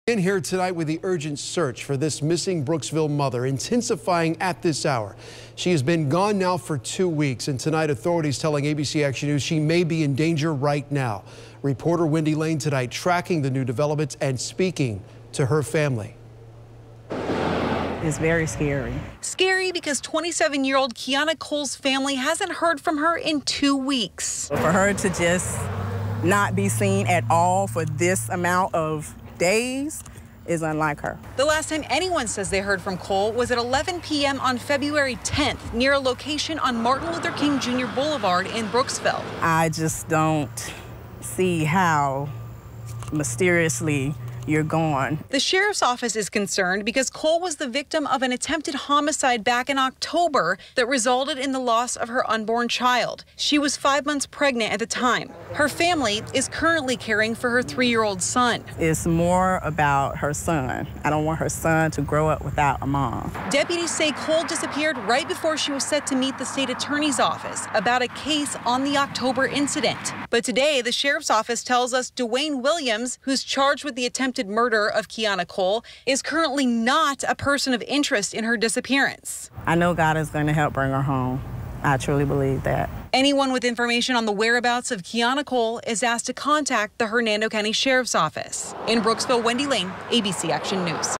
Deputies: Woman missing in Hernando County is previous attempted homicide victim
The Hernando County Sheriff's Office is searching for a missing woman who they say was previously the victim of an attempted homicide.